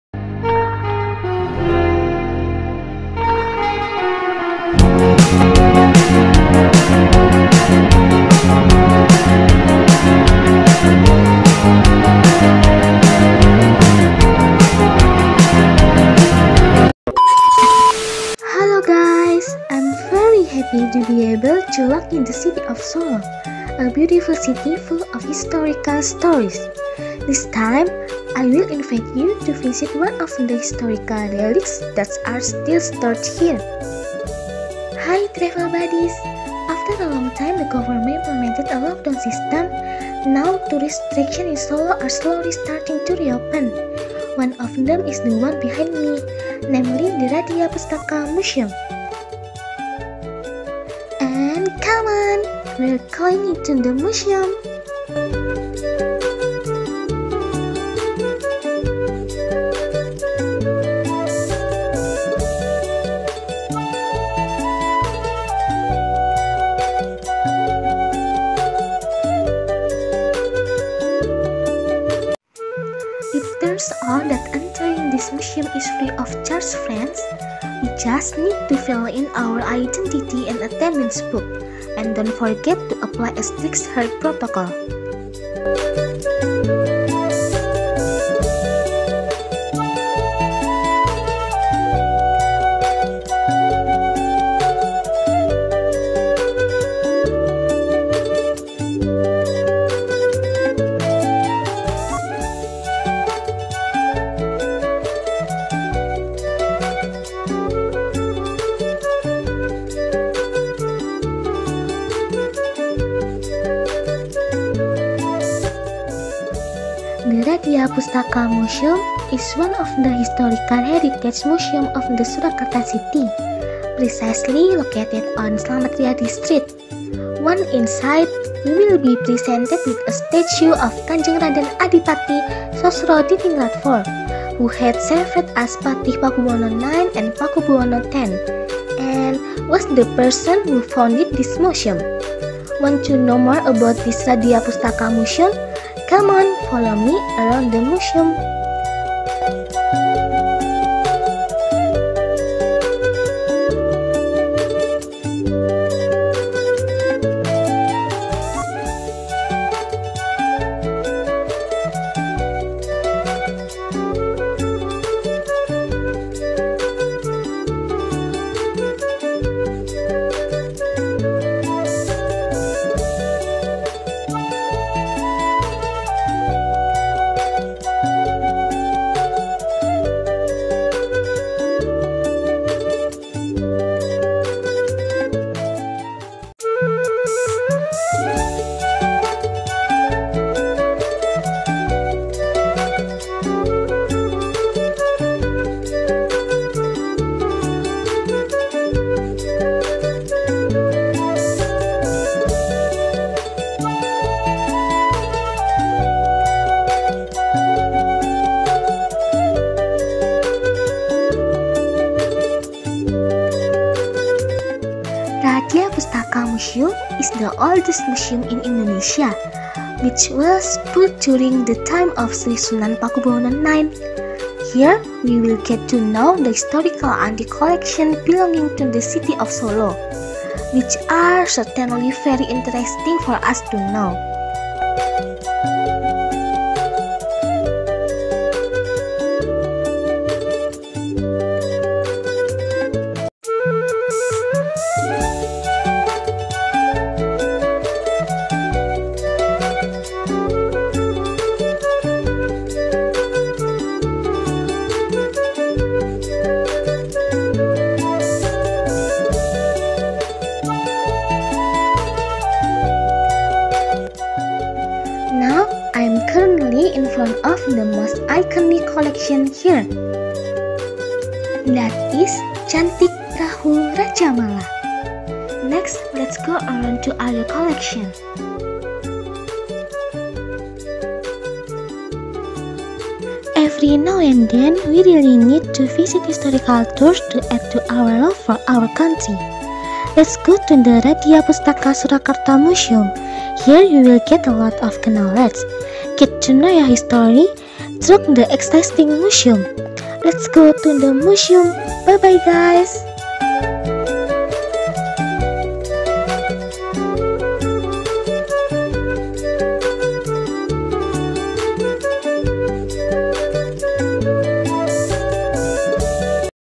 Museum Vlog - Radya Pustaka Museum Surakarta
hello travel buddies!

Let me introduce myself. My name is Nabila. In this video, I will share my excitement about visiting the Radya Pustaka Surakarta museum. Please watch till the end. Thank you to all who have wanted to.

This video is for an English course assignment
Sekolah Vokasi - Universitas Gadjah Mada - Bisnis Perjalanan Wisata

Nabila Saroja Estiyantara
21/479089/SV/19434